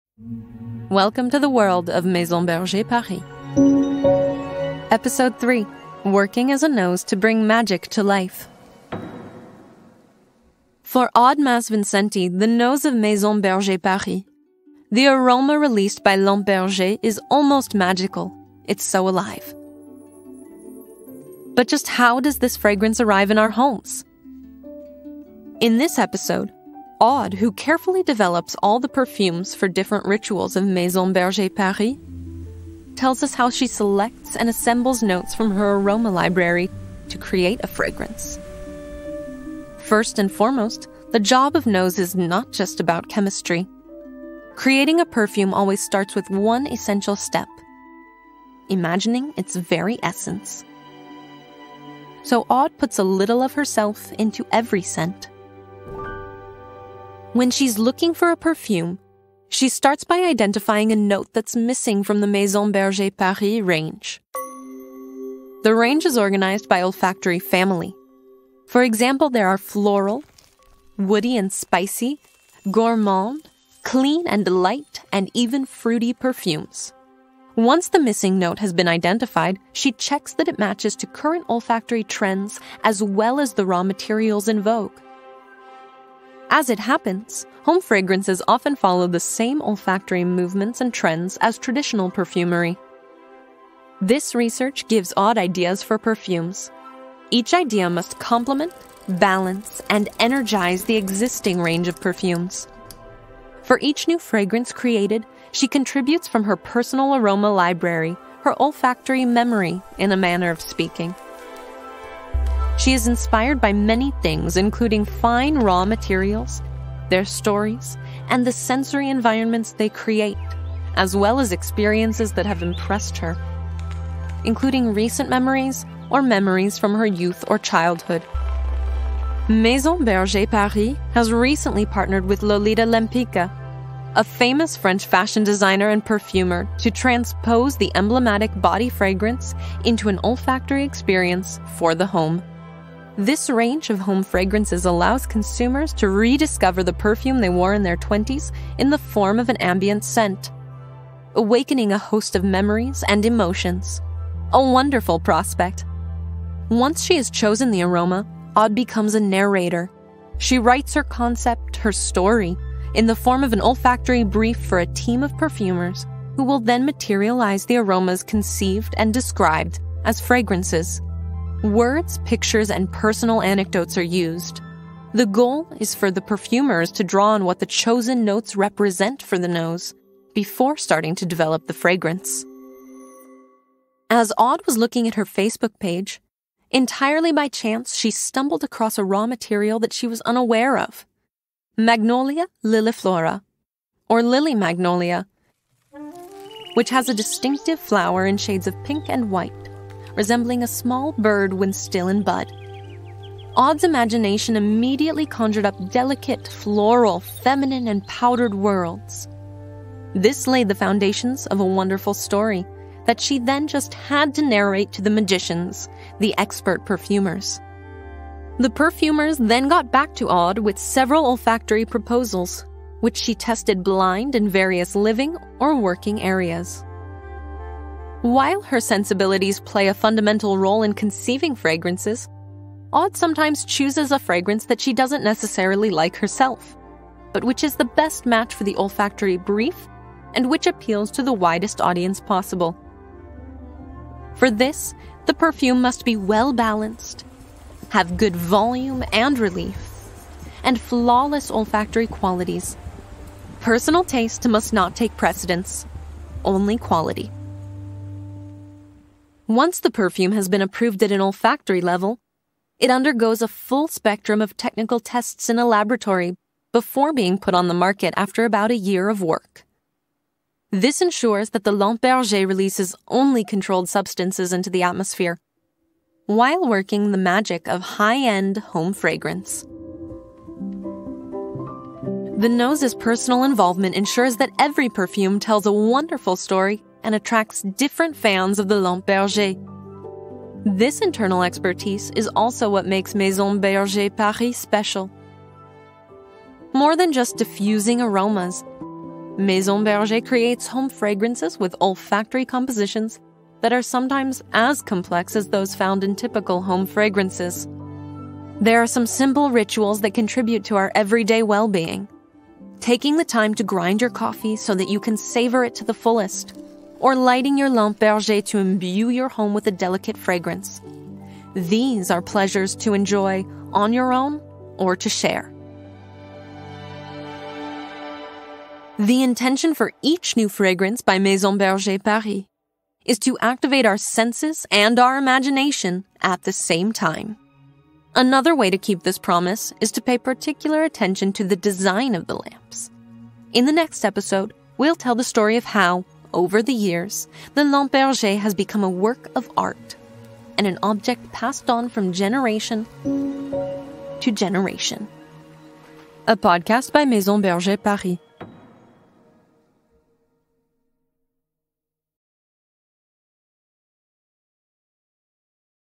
Podcast #3 - Working as a nose to bring magic to life
The nose of Maison Berger Paris tells you how she selects notes from her olfactory library to assemble these unique perfumes.